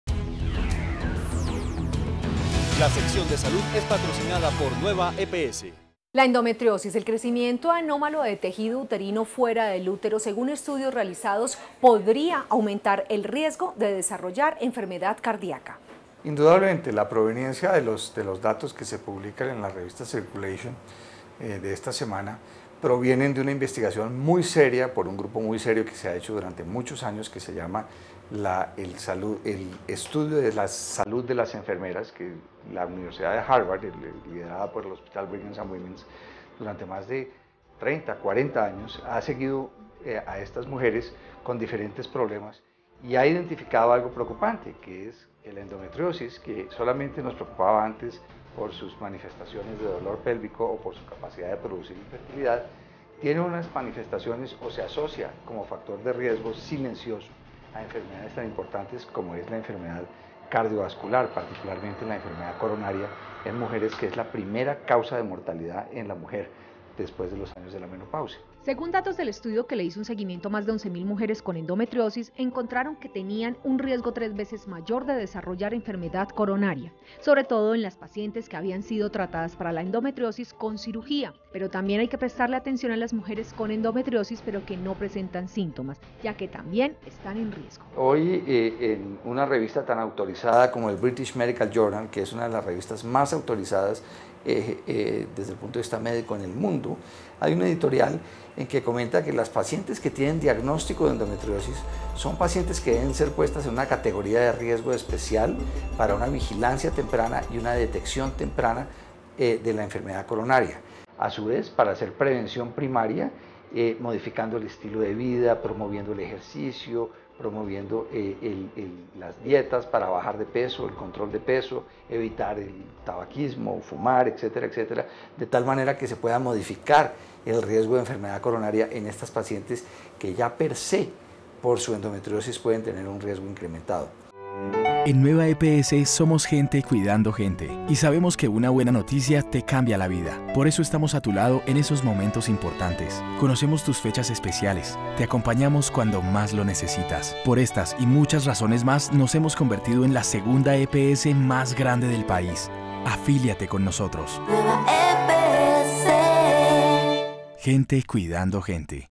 ASOCIACION ENTRE ENDOMETRIOSIS Y PROBLEMAS CARDIACOS
Estudio podria demostrar el alto riesgo de las pacientes con endometriosis a sufrir problemas cardiacos
Ricardo Rueda. especialista REPROTEC FSFB